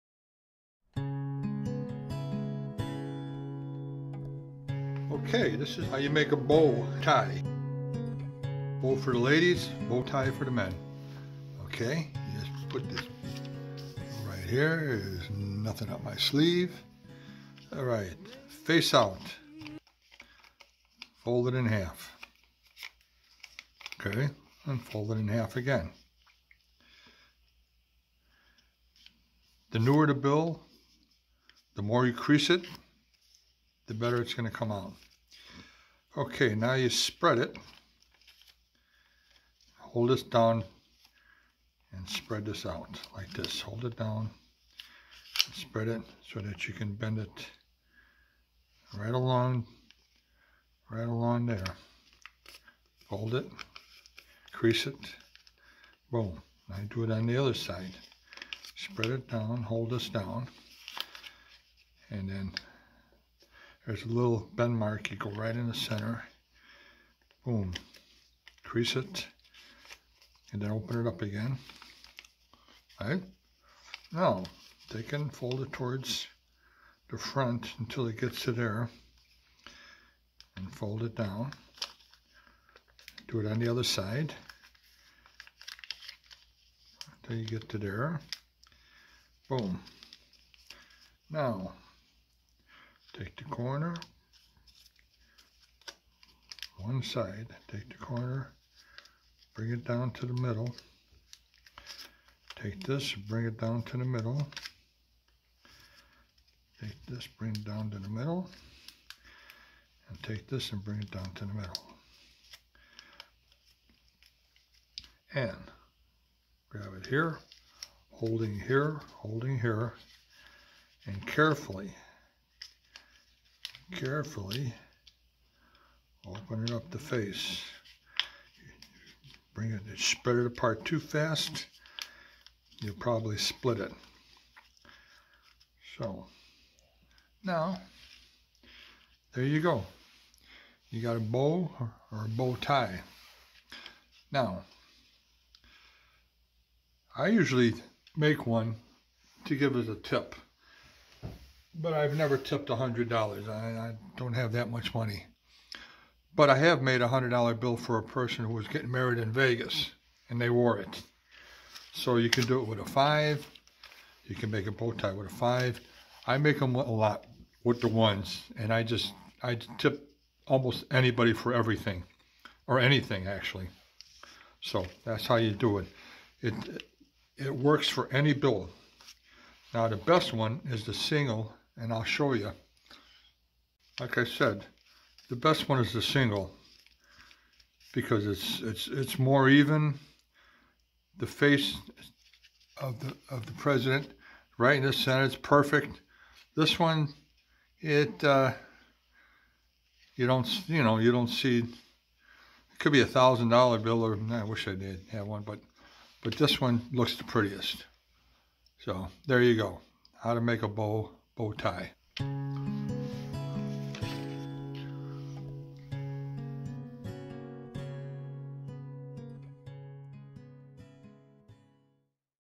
How to make a Dollar Bill Bow Tie
I made a $5 Bow Tie for the most adorable, tiniest person I ever met.
She loved it so much she asked how to make it. So, I made this video.